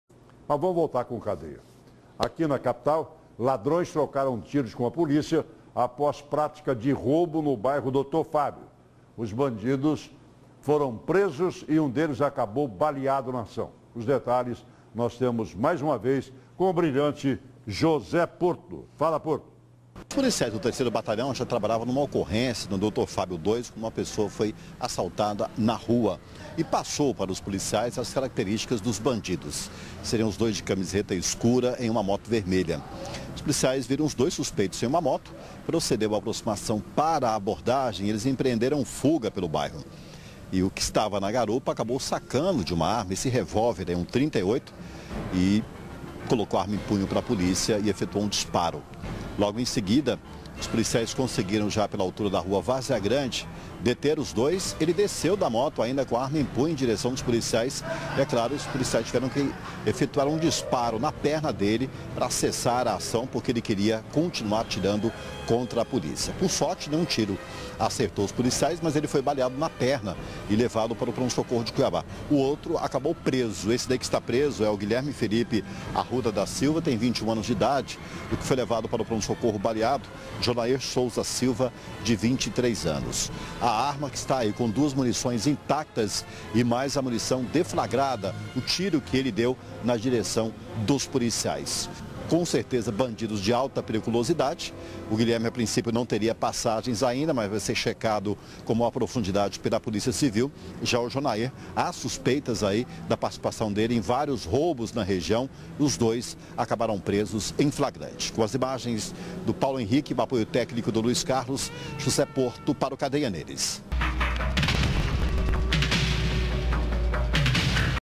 Bandidos trocam tiros com polícia após praticarem roubo
Tiroteio aconteceu no bairro Doutor Fábio em Cuiabá. Eles foram presos e durante a ação um foi baleado.